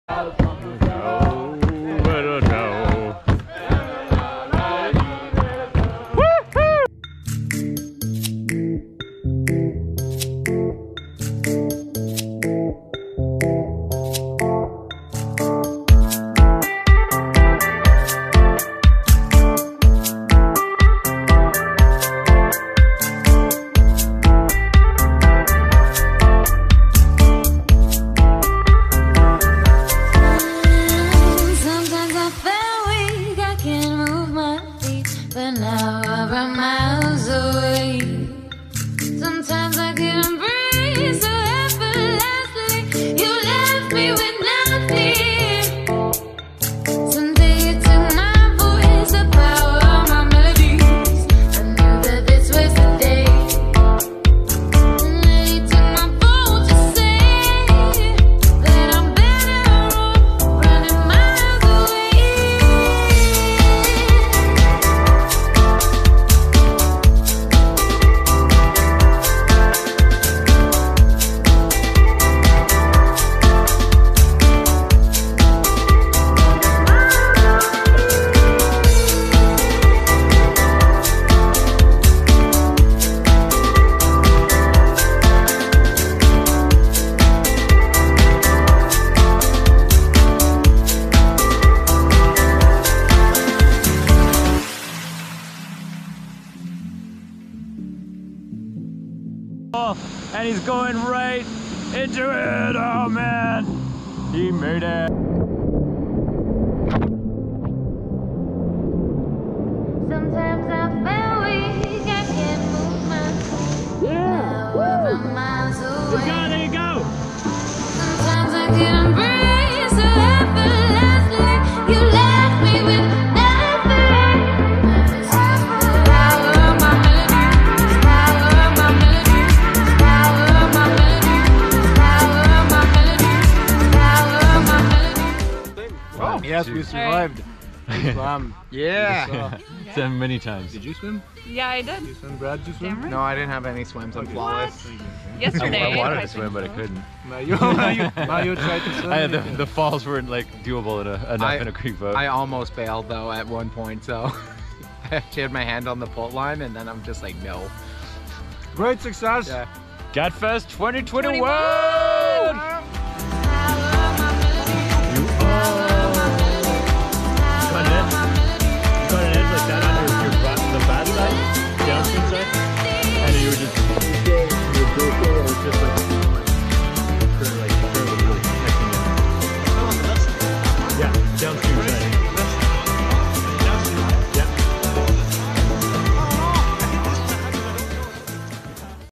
Festival Haute Gatineau
Big time fun at Gatineau festival, lots of carnage, lots of swims and lots of laughs.